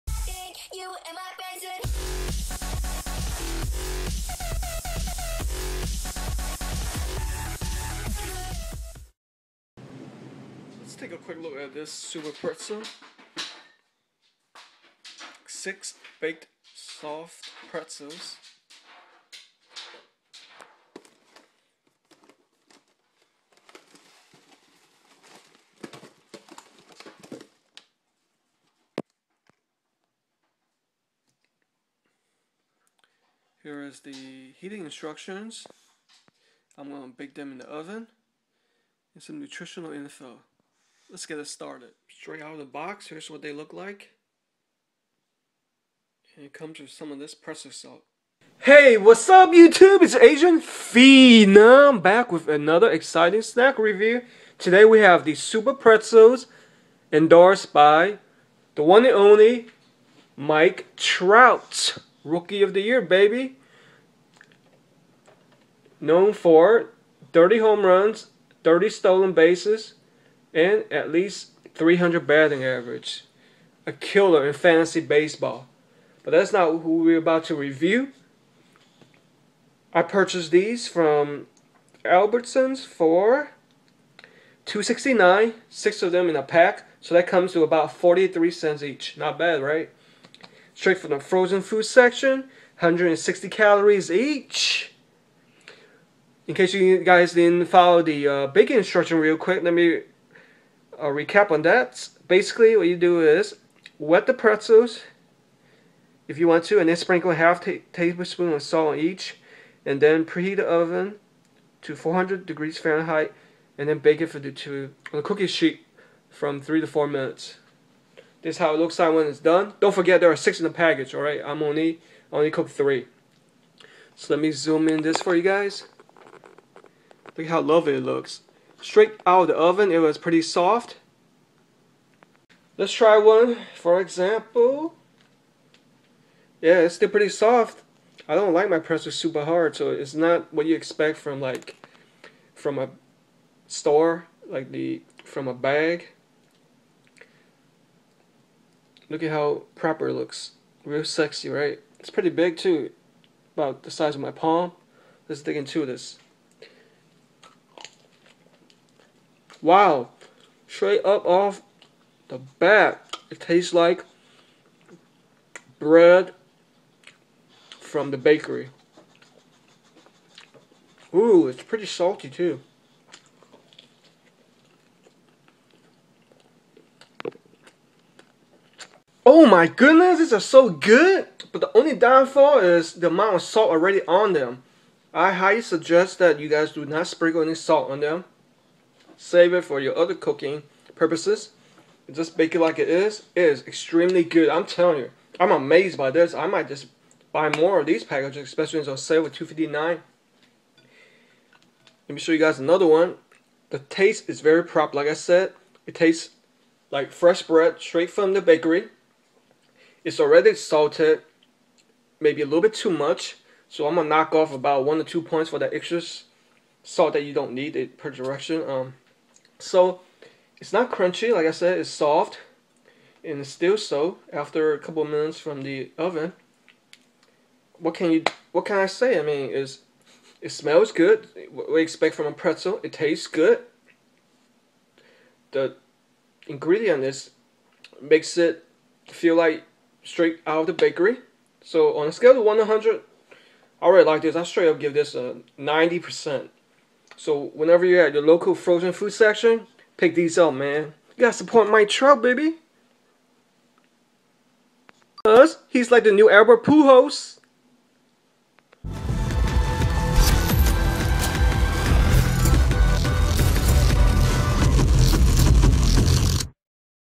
SuperPretzel (Original) Review
Trying the sexy & delicious SuperPretzel (Original) from the frozen food section.  I can't wait to try their Sweet Cinnamon SuperPretzels and the SuperPretzel PretzelDogs.